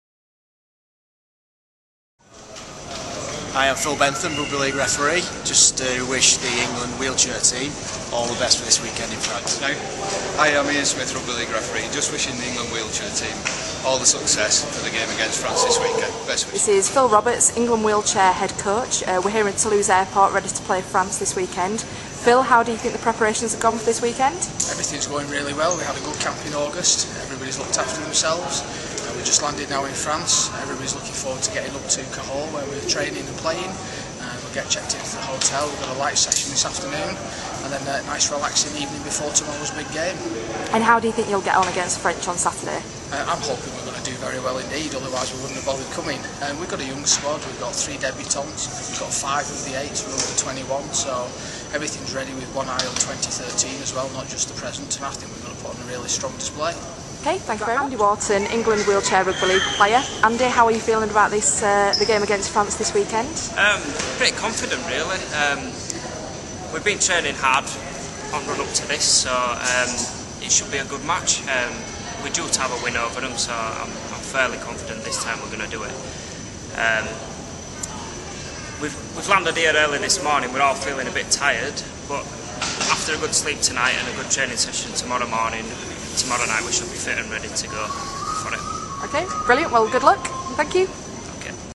England Wheelchair Rugby League team - Arrival at Toulouse Airport
England Wheelchair Rugby League arriving at Toulouse airport, to play France on Saturday 16th October 2010 in Cahors, France.

Messages from 2 Super league Match Officials Phil Bentham and Ian Smith, and messages from Head Coach Phil Roberts and player, Andy Wharton